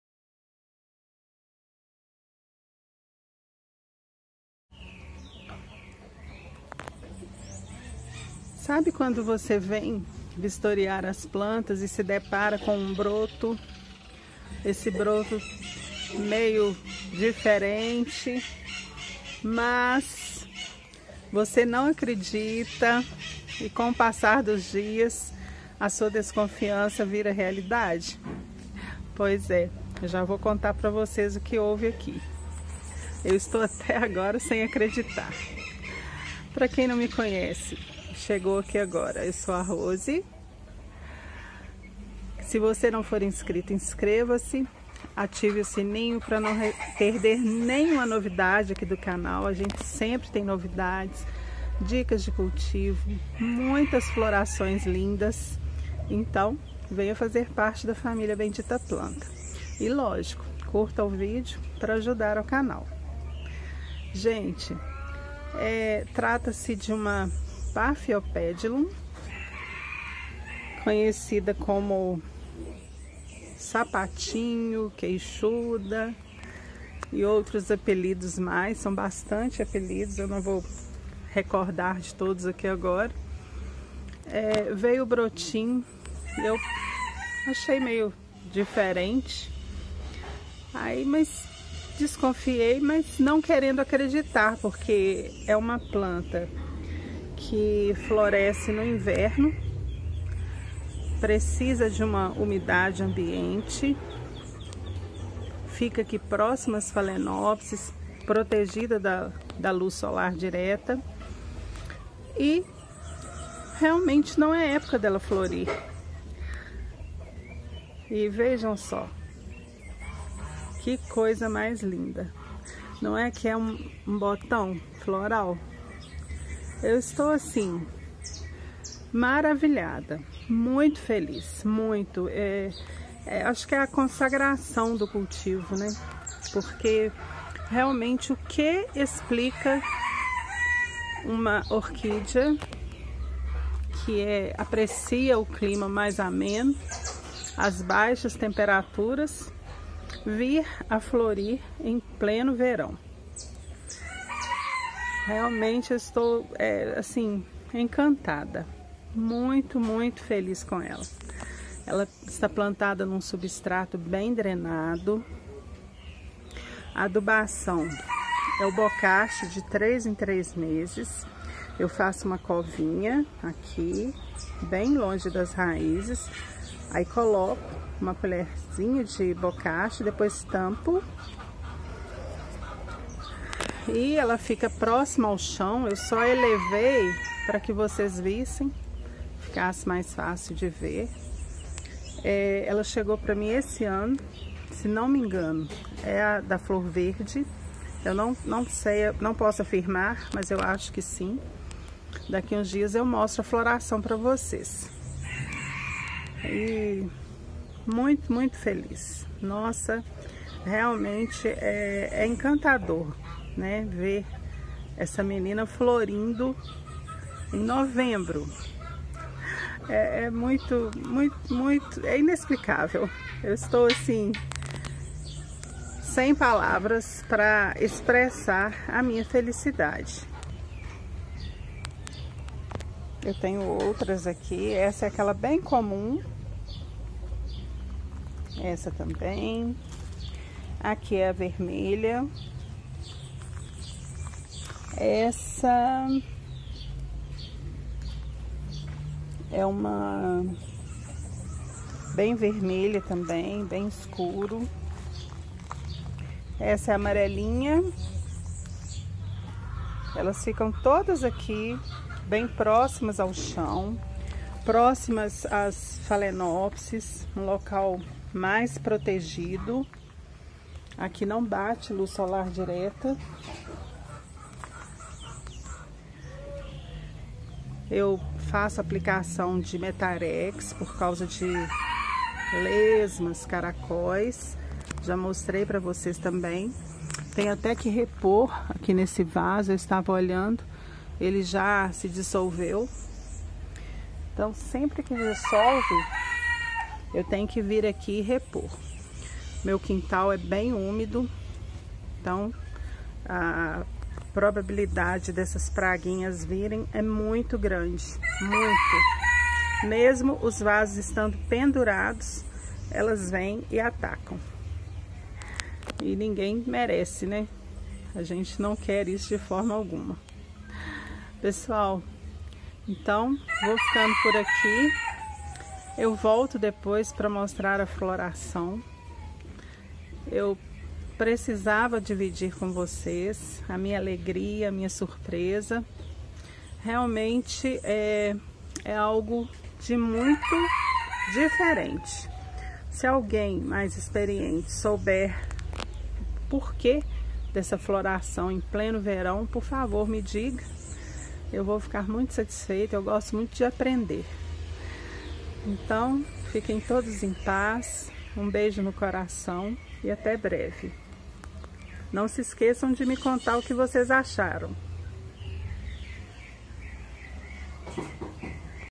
Surpresa no Orquidário 💚
Olá! Eu sou a Rose, sejam todos bem-vindos ao Bendita Planta. Aconteceu algo inesperado no orquidário, em pleno verão uma orquídea paphiopedilum, também conhecida como sapatinho irá florir. Vim dividir a minha felicidade com vocês.

Me sigam no instagram bendita_planta.